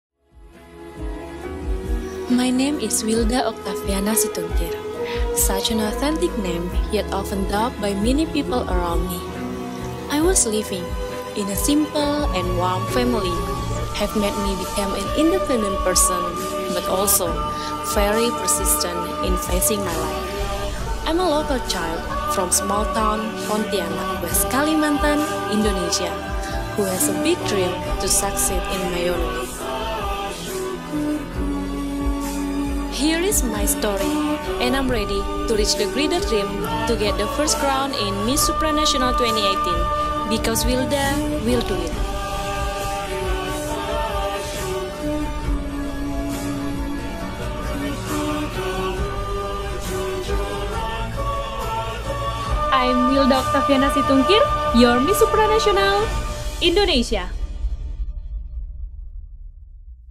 Miss Supranational Indonesia 2018 introduction video
Here is the introduction video of Wilda Octaviana Situngkir Miss Supranational Indonesia 2018. Don't miss it!